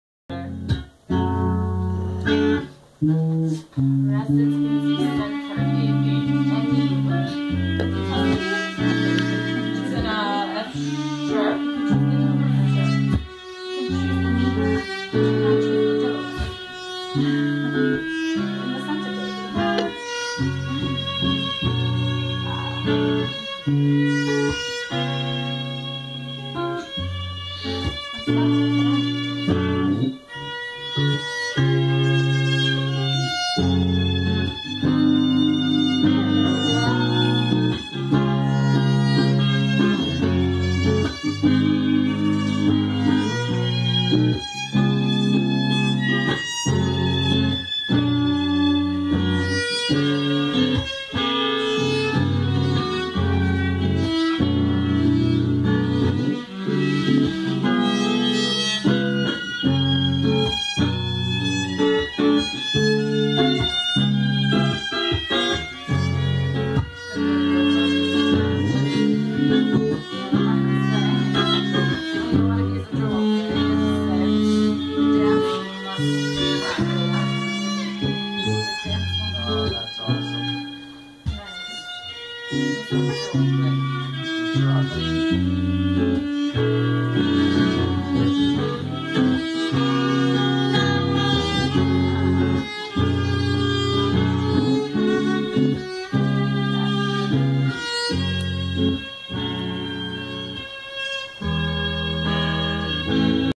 Jessica Joy Myers w/Ashes and Violets 9/27/14 🌼
Impromptu jam.  Sarah has a nice electric violin.  Still, its not mine and it didn't even have a shoulder rest.  Can't wait to play with them some more.